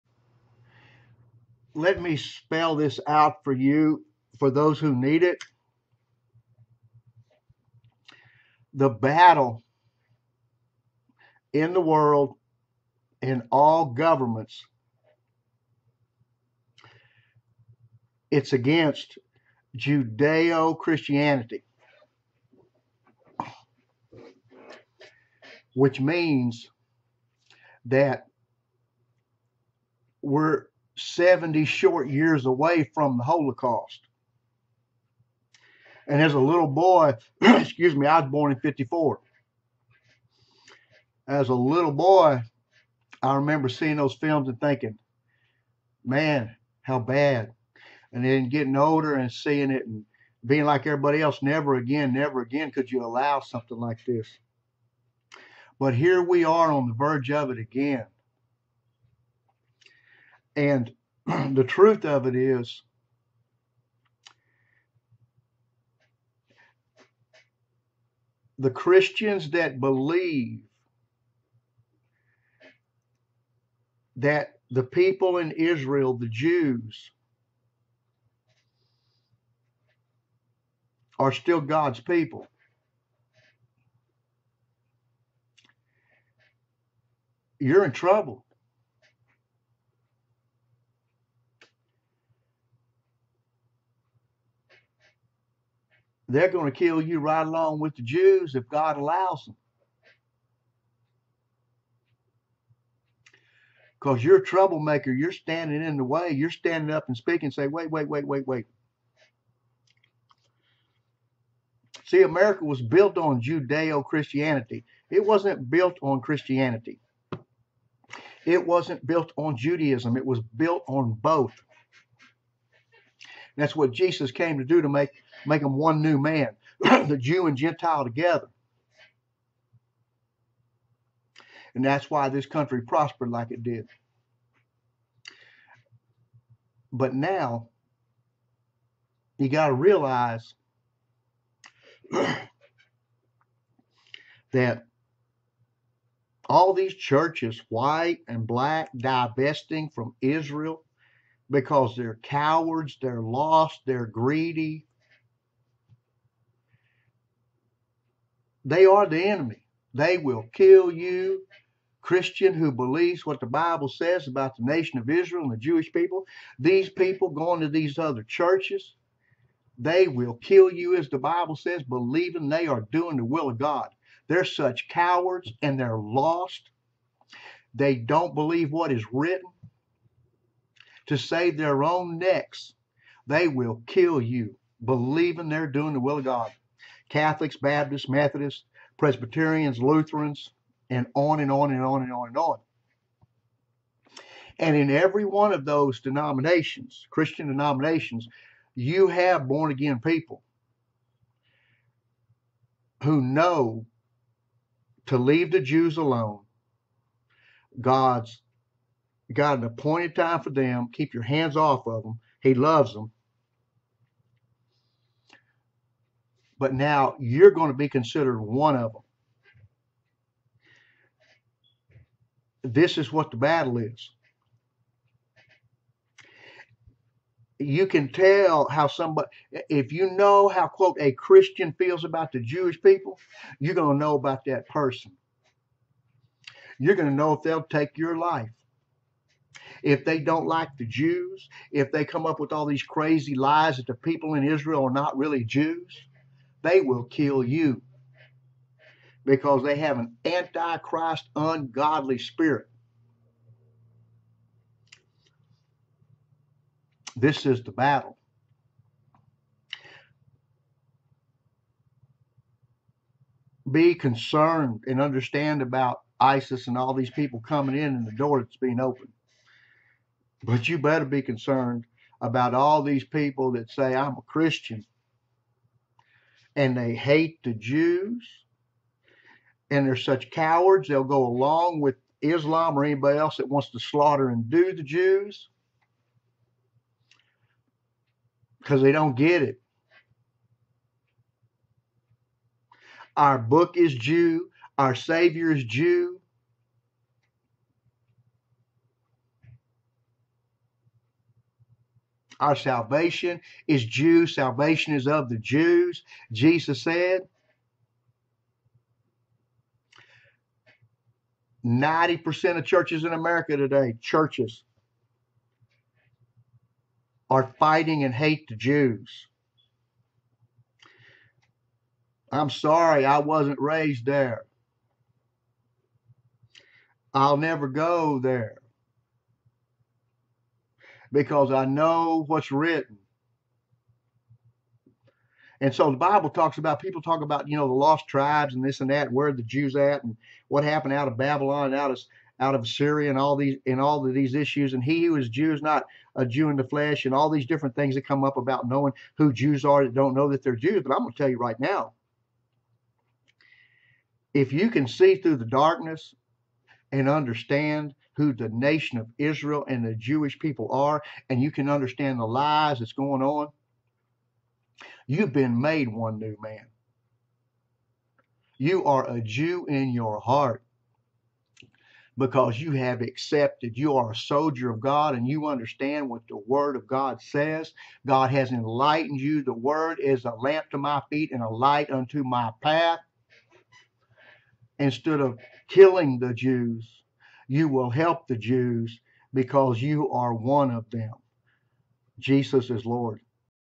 SALVATION OF THE JEWS!
JESUS MAKES THE JEW AND THE CHRISTIAN ONE NEW MAN IN HIM! JESUS SAID SALVATION IS OF THE JEWS.  IF IT IS NOT JUDEO-CHRISTIAN IT IS NOTHING! EVIL CHURCH GOERS DIVEST FROM ISRAEL, AND ARE WITH ISLAM HATING THE JEWS,AND BOTH WILL KILL CHRISTIANS THAT LOVE THE JEWISH PEOPLE.